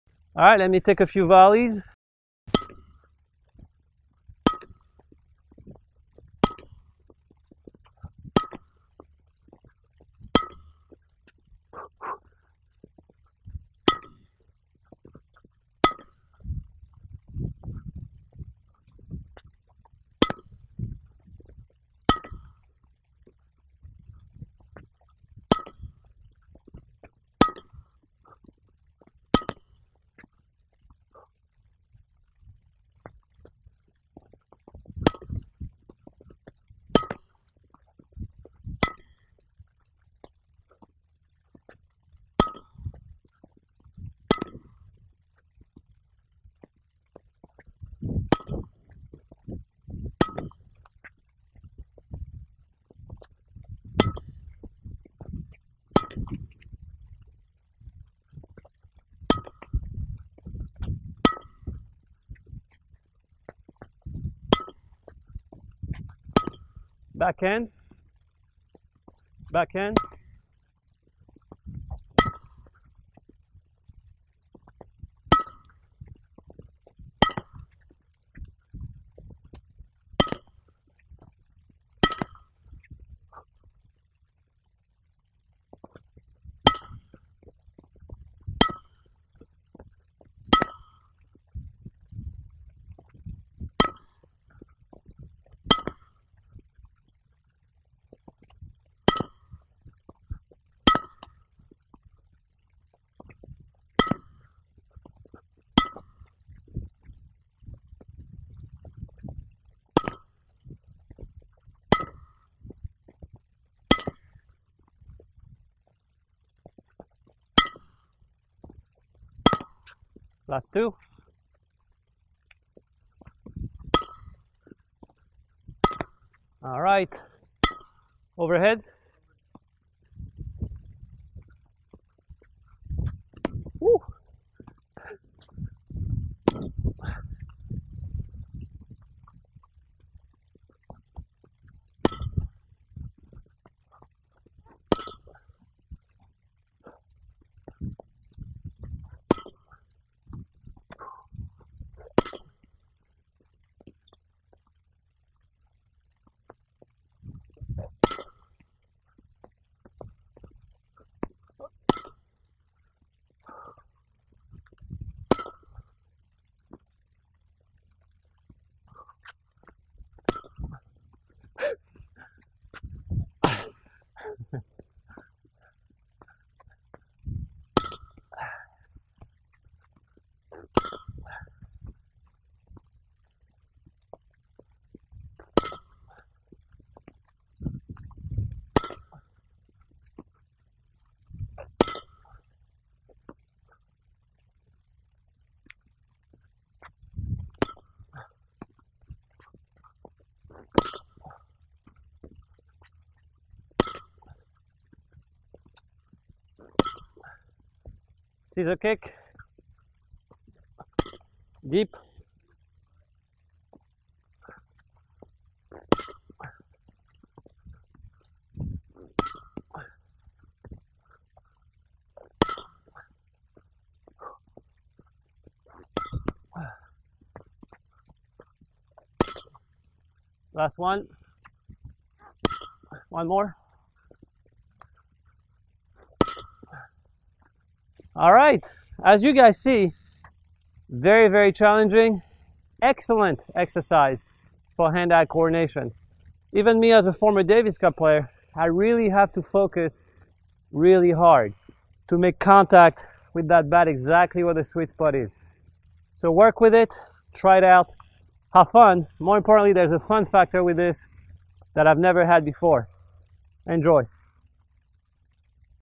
Net Game
I love to practice volleys with the Sweet Spotter because after 5 to 10 minutes of connecting with my hand eye coordination, I just do not seem to be able to miss a shot with my racket.  Don't let the stray balls fool you; each time you make contact with the Sweet Spotter, you are getting closer and closer to perfection.